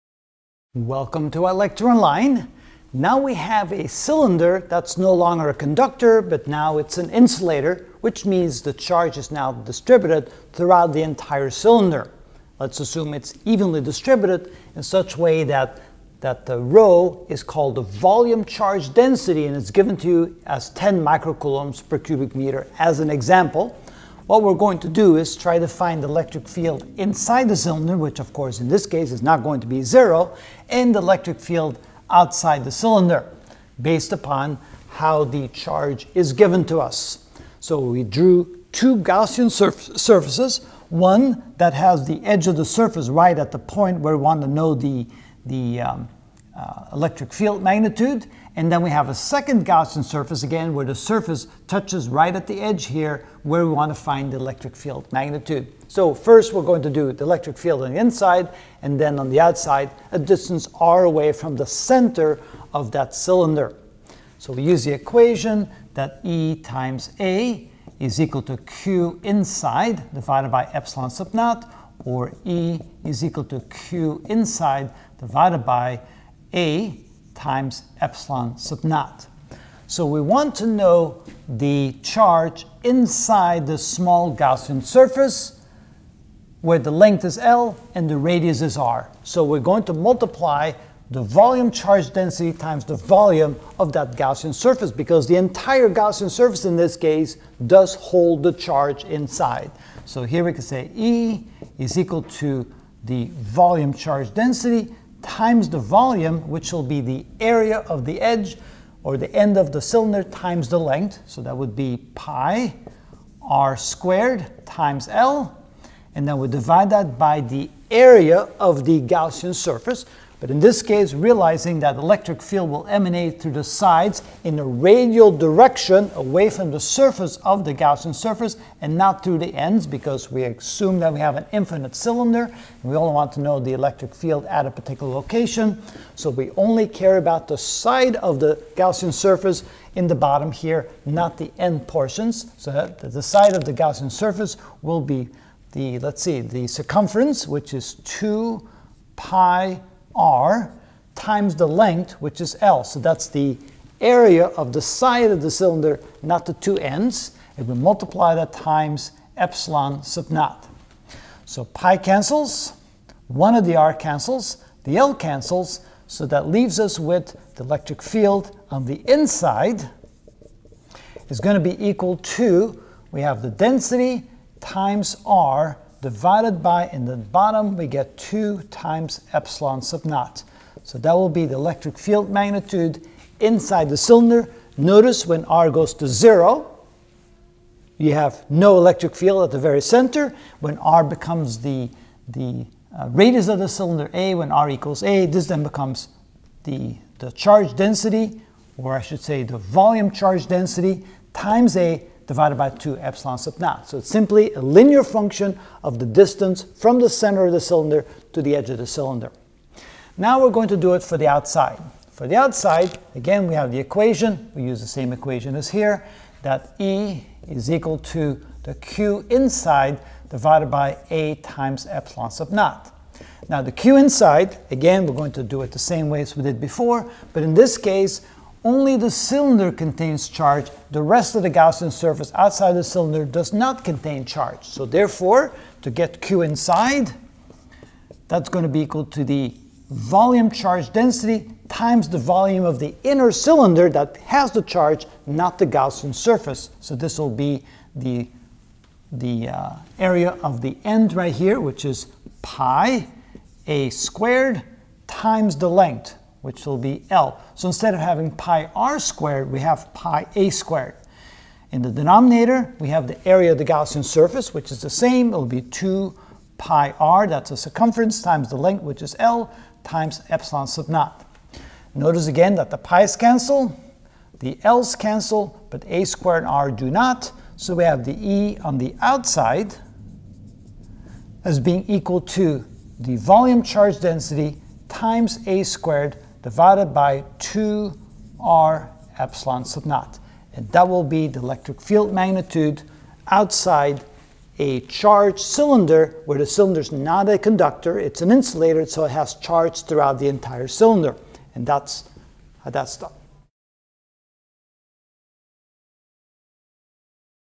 Physics 37.1 Gauss's Law Understood (28 of 29) Infinite Cylinder of Charge 3
In this video I will find the electric field E=? a distance from an infinite cylinder (cylinder is a INSULATOR, charges evenly distributed inside) of charge where the VOLUME charge density is given.